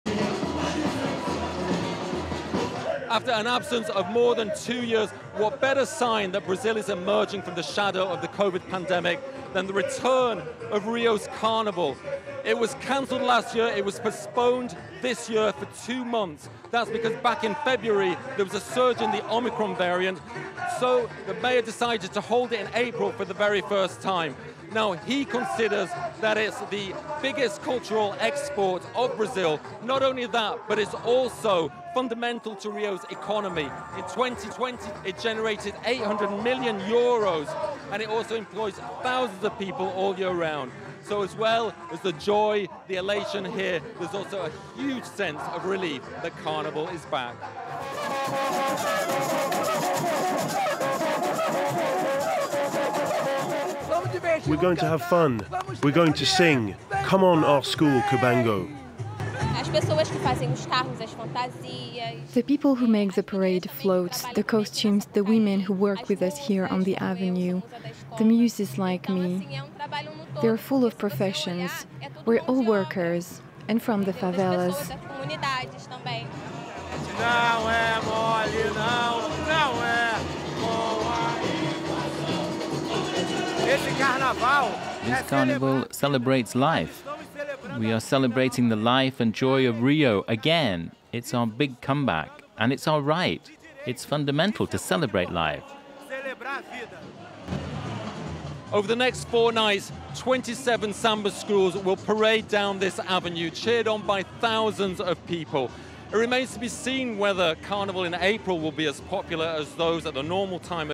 Brazil: Rio's Carnival parade is back • FRANCE 24 English
After two bleak years of lock-downs and loss, Rio de Janeiro will hold its famed carnival this weekend for the first time since Covid-19 hit Brazil, promising a giant, glittering spectacle of pandemic catharsis. FRANCE 24's Jan Onasko tells us more.